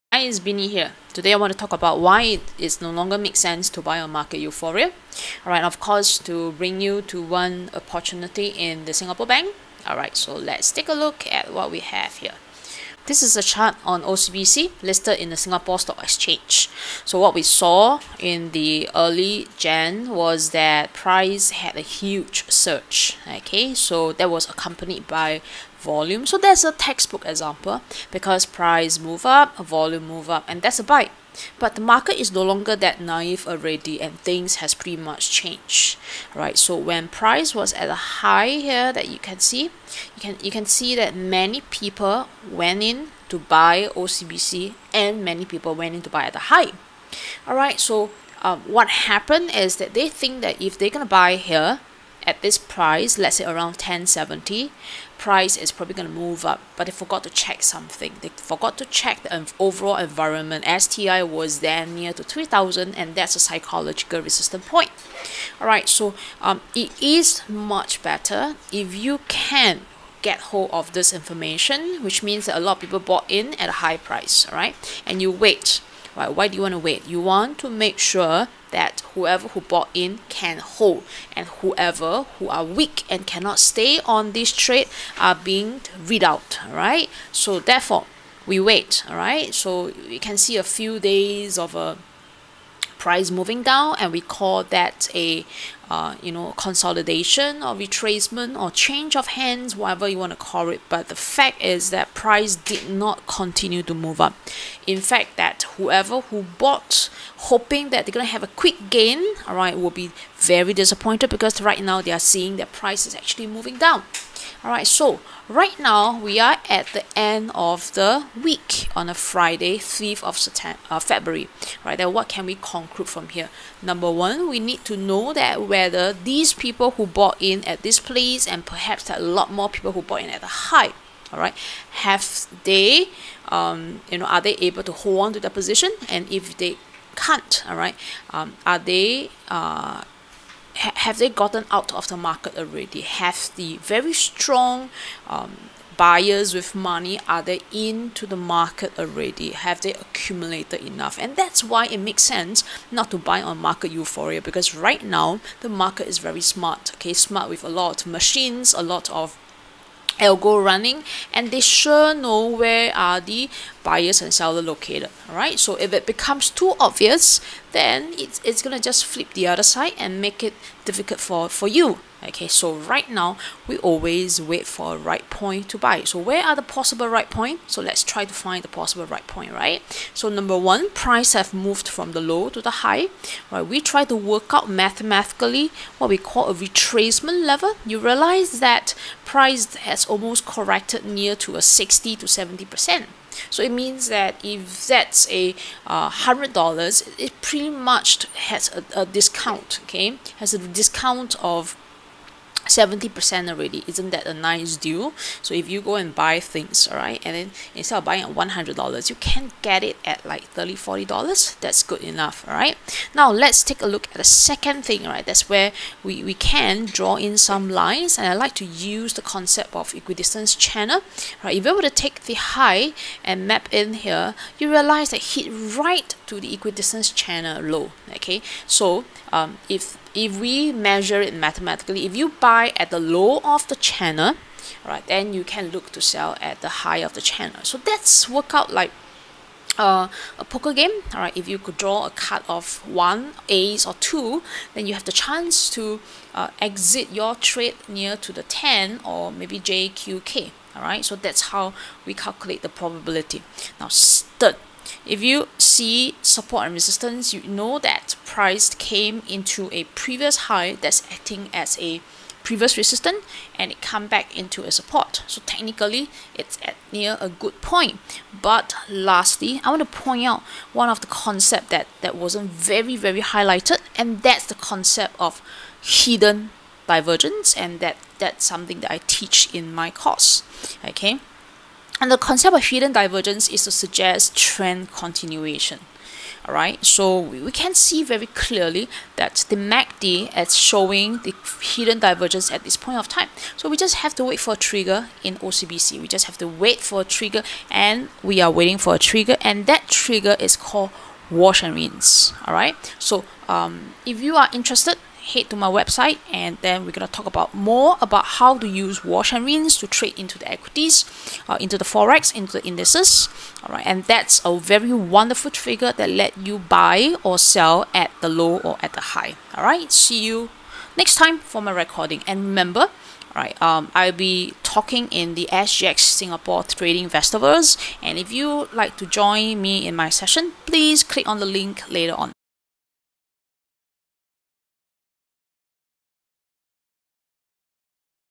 Why it doesn't make sense now to trade on market euphoria (aka breakout trade)? OCBC illustrated
The trading condition now is very different vs decades ago with algos and AI that know inside out about key levels and your positions.

Instead of moving in the usual expected manner, a very clear and obvious movement is often delayed to create stress on your trades. This is why in the new world of trading, it doesn't make sense to trade on most breakouts (yes, I trade only on ONE type of high probability breakout only).

In this video, you will understand why breakout trades don't really work now as trading range is easily exhausted with superb market intelligence.

For a higher probability of winning, use the 3 ways that I have presented to determine good zone of entry.
I have illustrated using OCBC on how we plan for our trades. But you need to have a trigger. Yes, that's wash and rinse. No wash and rinse, no trigger.

Watch to see if the OCBC trade pans out as said.